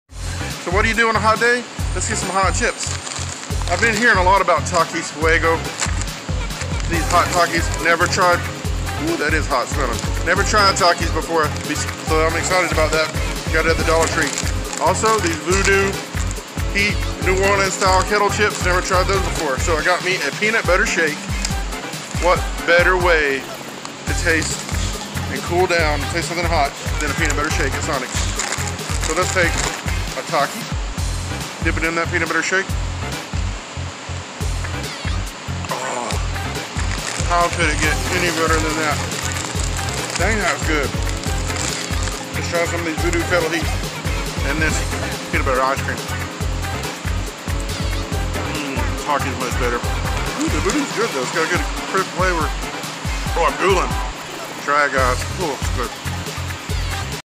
BEAT the HEAT!
Trying to BEAT THE HEAT and cool off while trying something HOT?  Take some TAKIS FUEGO and Zapp's Voodoo HEAT New Orleans kettle style potato chips and dip them in a peanut butter handmade milkshake from SONIC and it is yummy GOOD!!

GOD BLESS you and yours each and every day from the BIG FLUFFY TALKING MACHINE!  Enjoy the sunshine on your face and the wind at your back!  :))

Hey BRAiN StORMers ! In case you are interested sharethepositive merch is now available!